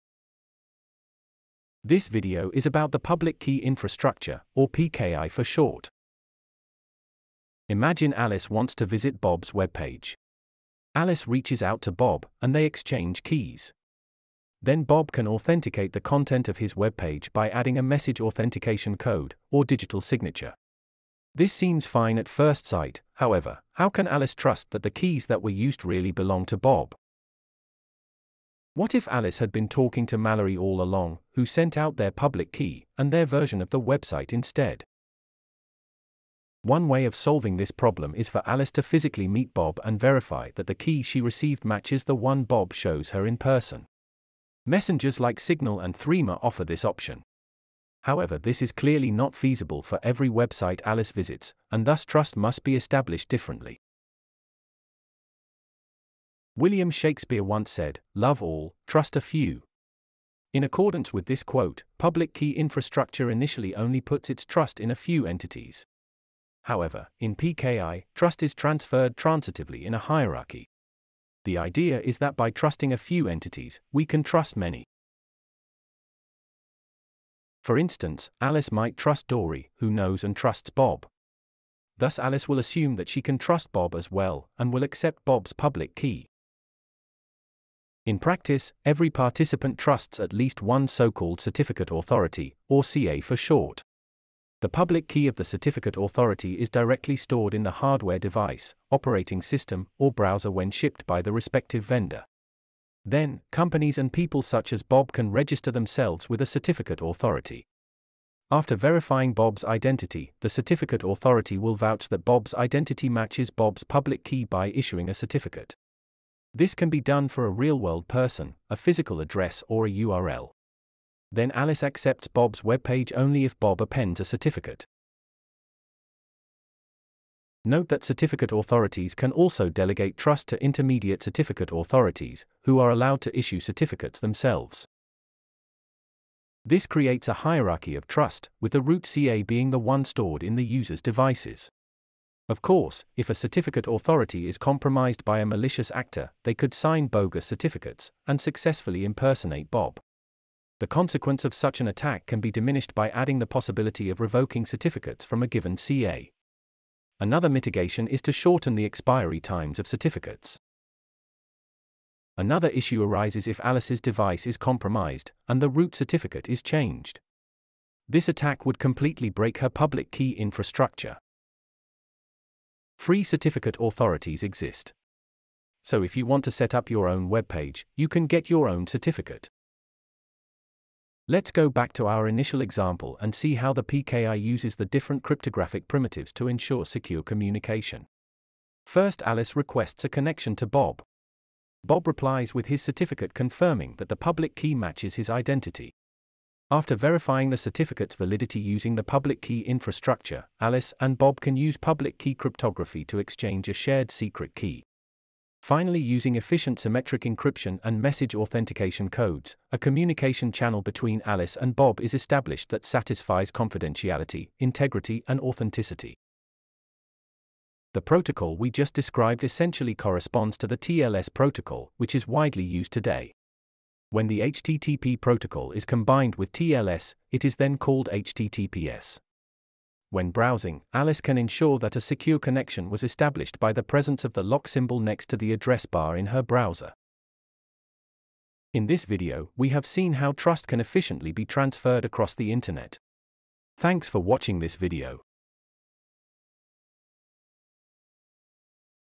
Public Key Infrastructure
In this video, we discuss how the cryptographic algorithms work together in the TLS framework. We also study why you can trust your web browser to know that you are really on your bank’s web page, and not some dangerous counterfeit web page.